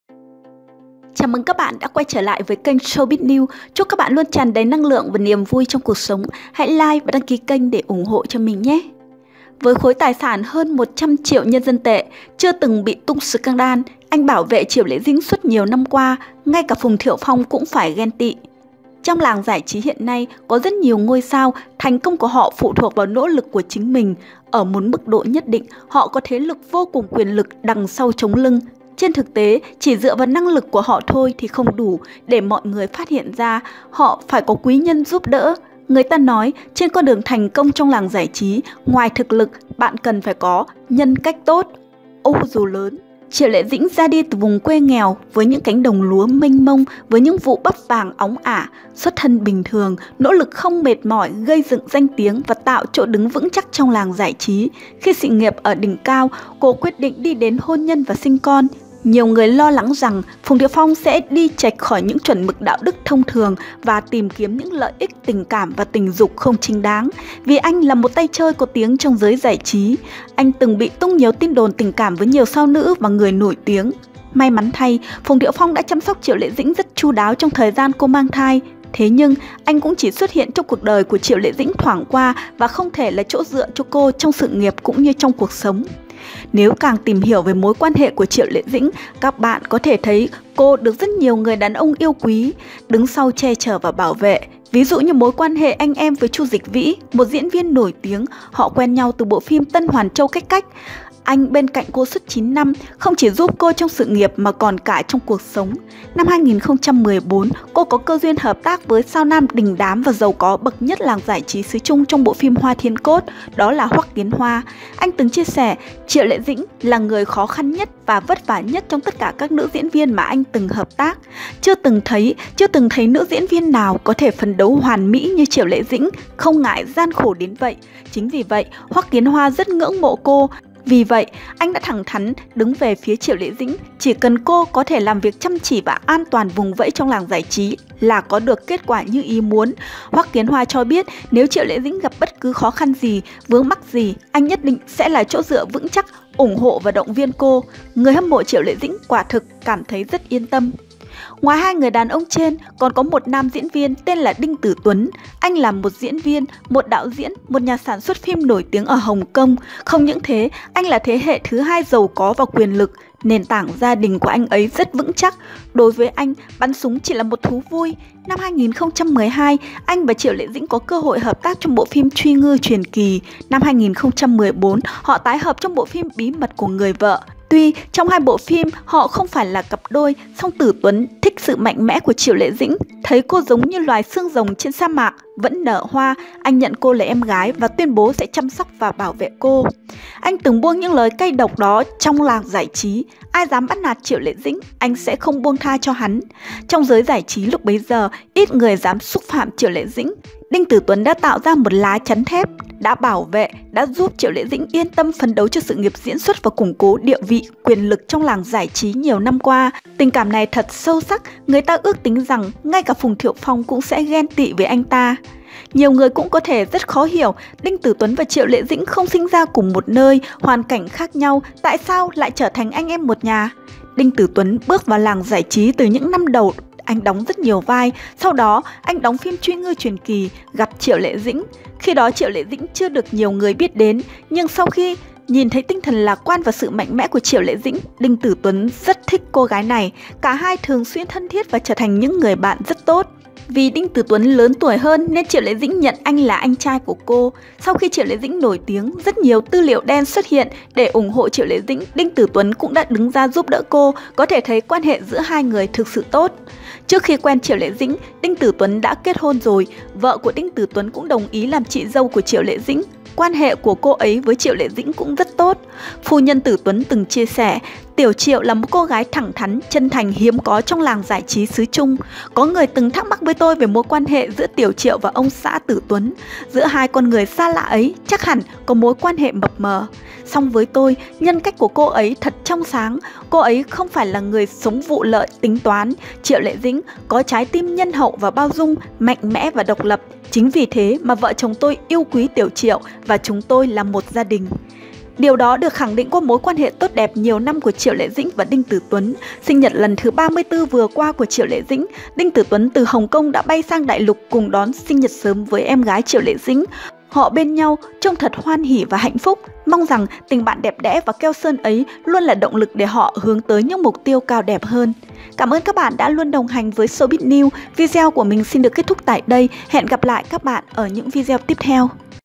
Vợ tài tử Hồng Kông nói: "Tiểu Triệu và chồng tôi không có sự mập mờ, chúng tôi là 1 gia đình, "
Nhiều người cũng có thể rất khó hiểu Đinh Tử Tuấn và Triệu Lệ Dĩnh không sinh ra cùng một nơi, hoàn cảnh lại khác nhau, tại sao lại trở thành anh em một nhà?
Đinh Tử Quân bước vào làng giải trí từ những năm đầu đóng nhiều vai, sau đó khi đóng phim Truy ngư truyền kì, anh gặp Triệu Lệ Dĩnh. Khi đó, Triệu Lệ Dĩnh chưa được nhiều người biết đến, nhưng sau khi nhìn thấy tinh thần lạc quan và mạnh mẽ của Triệu Lệ Dĩnh, Đinh Tử Tuấn đã rất thích cô gái này . Cả hai thường xuyên thân thiết và trở thành những người bạn rất tốt, vì Đinh Tử Tuấn lớn tuổi hơn nên Triệu Lệ Dĩnh nhận anh là anh trai của cô.
Sau khi Triệu Lệ Dĩnh nổi tiếng, rất nhiều tư liệu đen đã xuất hiện, để ủng hộ Triệu Lệ Dĩnh, Đinh Tử Tuấn cũng đứng ra giúp đỡ cô, có thể thấy quan hệ giữa hai người thực sự rất tốt.
Trước khi quen Triệu Lệ Dĩnh, Đinh Tử Tuấn đã kết hôn rồi, vợ của Đinh Tử Tuấn cũng đồng ý làm chị dâu của Triệu Lệ Dĩnh, quan hệ của cô ấy với Triệu Lệ Dĩnh cũng rất tốt. Phu nhân Tử Tuấn từng chia sẻ: Tiểu triệu là một cô gái thẳng thắn, chân thành hiếm có trong làng giải trí xứ Trung. Có người từng thắc mắc với tôi về mối quan hệ giữa tiểu Triệu với ông xã Tử Tuấn, giữa 2 con người xa lạ ấy chắc hẳn có mối quan hệ mập mờ. Song với tôi, nhân cách của cô ấy thật trong sáng, cô ấy ko phải là người sống vụ lợi, tính toán. Cô ấy có trái tim nhân hậu và bao dung, mạnh mẽ và độc lập, chính vì thế mà vợ chồng tôi yêu quý tiểu triueej, và chúng tôi là 1 gia đình.